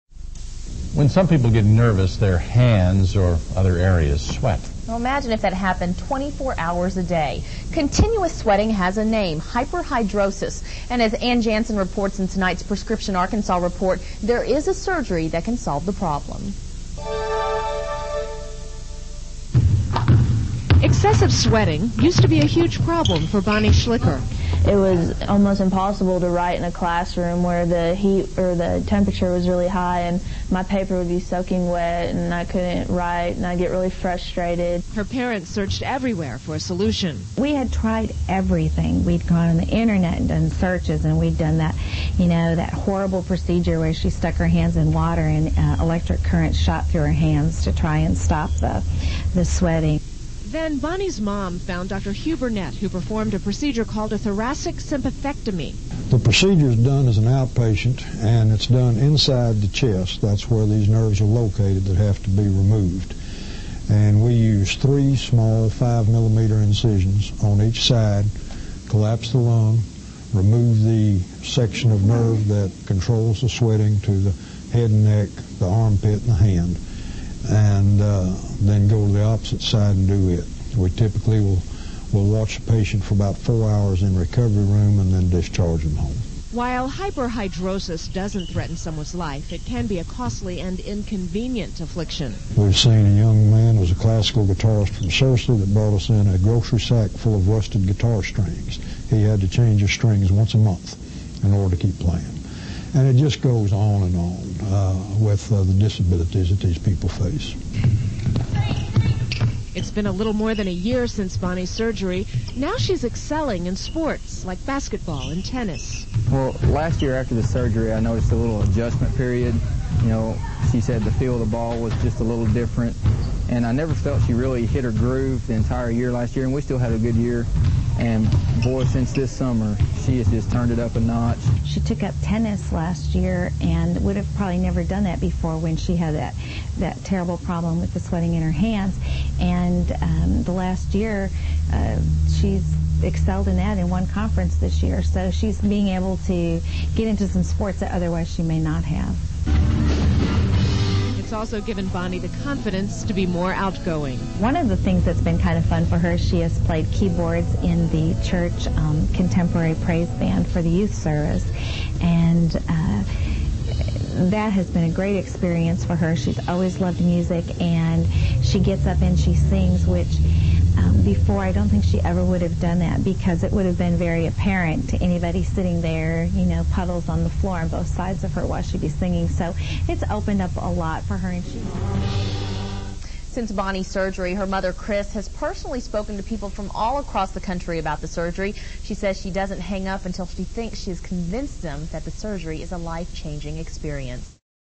Problems with sweat? Surgery for Hyperhidrosis may help.
For those with excessive sweating, called Hyperhidrosis, surgery may be the answer.